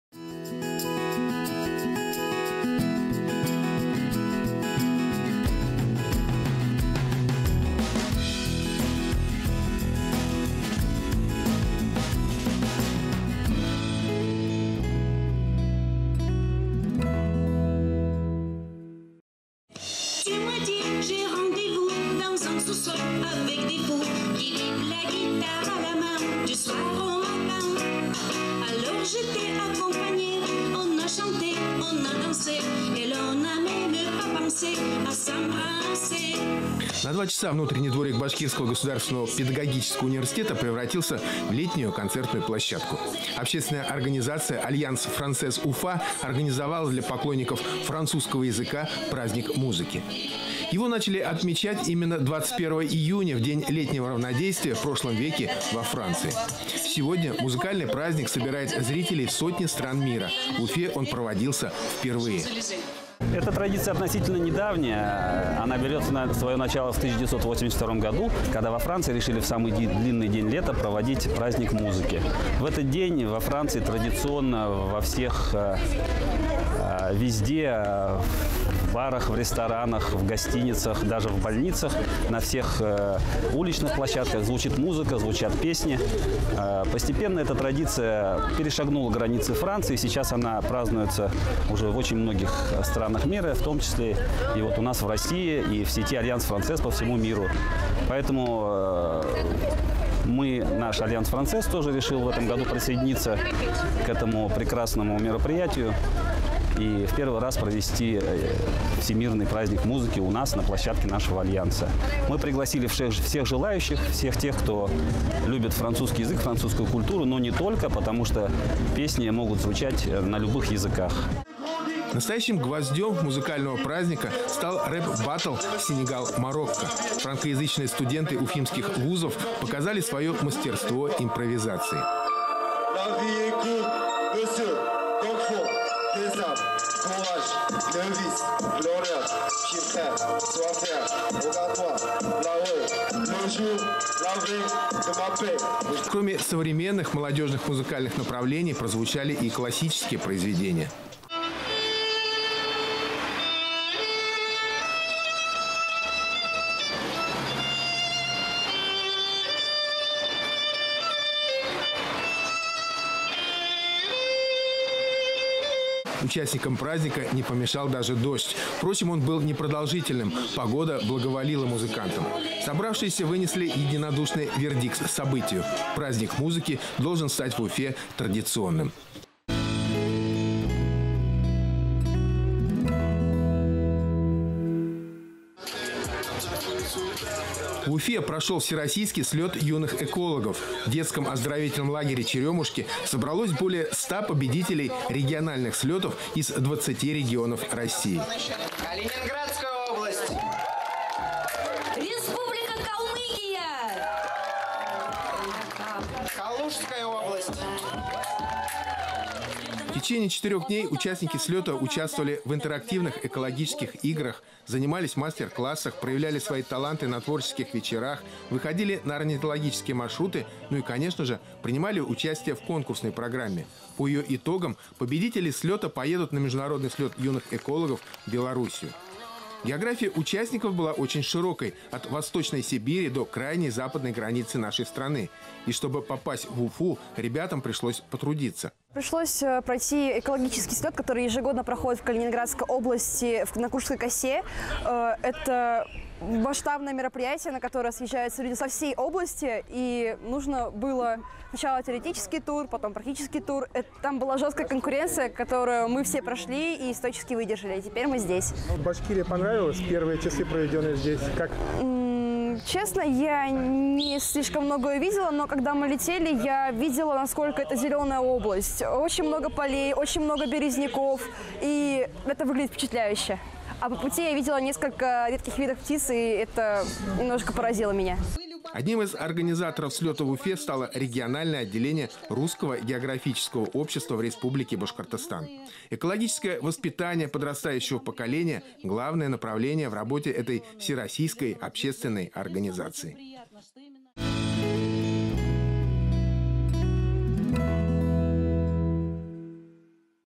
Моя Планета - Башкортостан
Праздник музыки в Уфе (Альянс Франсез-Уфа), Всероссийский слет юных экологов.